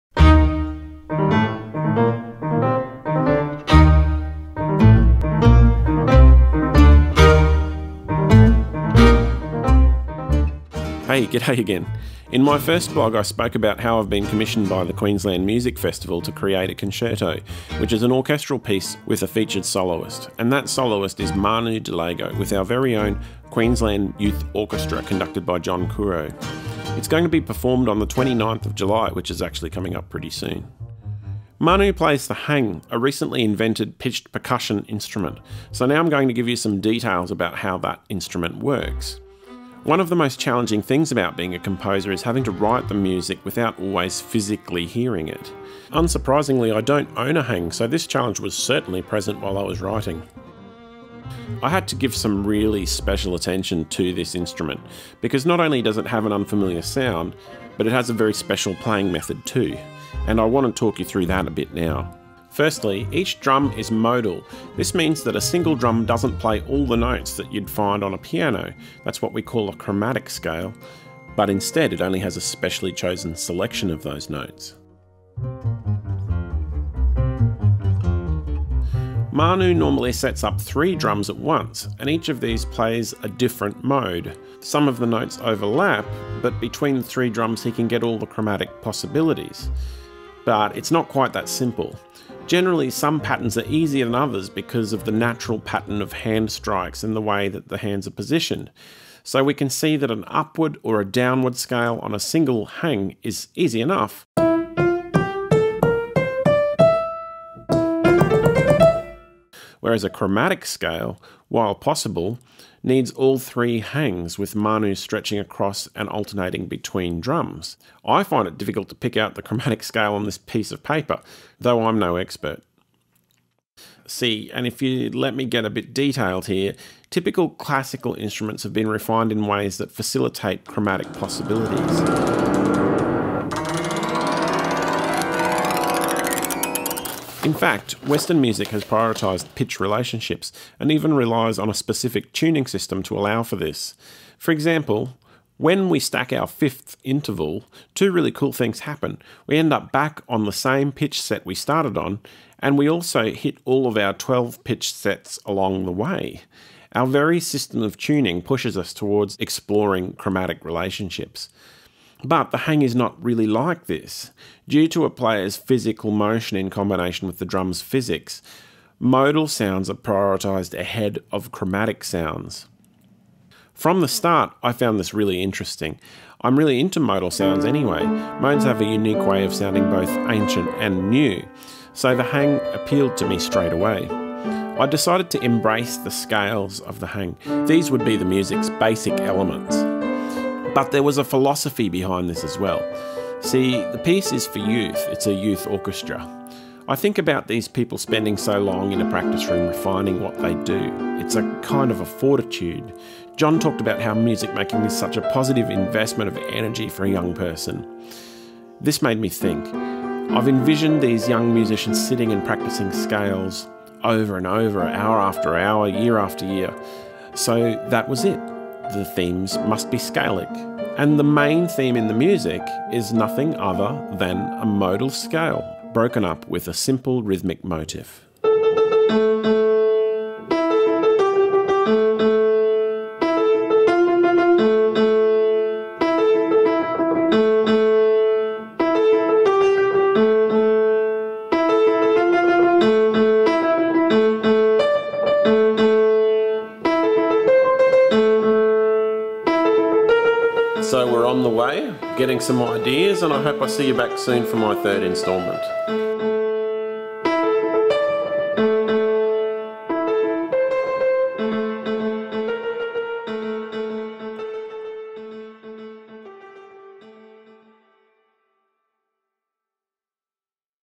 Hang with QYO | Dr Thomas Green Video Blog 2
Queensland composer Dr Thomas Green talks us through the finer points of composing for hang - an instrument invented only a decade ago! Catch Thomas' world premiere composition, with hang played by international star percussionist for Bjork, Manu Delago at Hang with QYO, part of Queensland Music Festival.